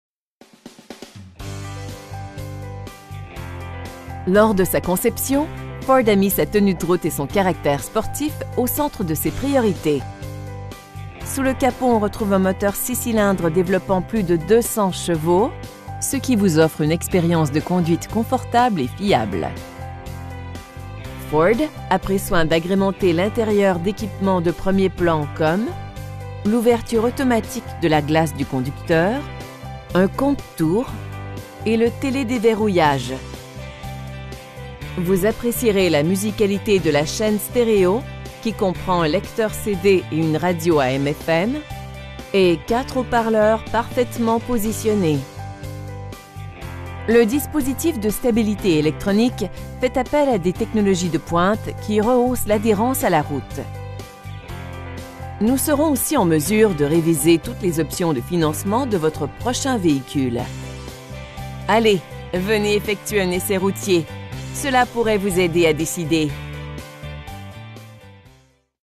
2010 Ford Mustang V6, MAN, CUIR, SIEGES CHAUFF, PIRELLI
Brossard Hyundai
8750, Boul Taschereau

La Ford Mustang 2010. Ce coupé à 4 passagers affiche moins de 90 000 km au compteur. Lors de sa conception, ford a mis sa tenue de route et son caractère sportif au centre de ses priorités. Sous le capot on retrouve un moteur 6 cylindres développant plus de 200 chevaux, ce qui vous offre une expérience de conduite confortable et fiable. Ford a pris soin d'agrémenter l'intérieur d'équipement de premier plan, comme: l'ouverture automatique de la glace du conducteur, un compte-tours, des essuie-glaces intermittents, climatiseur, colonne de direction inclinable, régulateur de vitesse, et le télédéverrouillage. Vous apprécierez la musicalité de la chaîne stéréo, qui comprend une lecteur CD et une radio AM/FM, et 4 haut-parleurs parfaitement positionnés. Ford accorde la priorité à la sécurité et la protection en incluant: coussins gonflables à l'avant avec détecteur de poids du passager, coussins gonflables latéraux à l'avant, système antipatinage, système d'assistance au freinage, un avertisseur d'urgence, et freins à disque aux 4 roues avec antiblocage. Le dispositif de stabilité électronique fait appel à des technologies de pointe qui rehaussent l'adhérence à la route. Nous serons aussi en mesure de réviser toutes les options de financement de votre prochain véhicule. Nous serons heureux de répondre à toutes vos questions. Allez, venez effectuer un essai routier, cela pourrait vous aider à décider!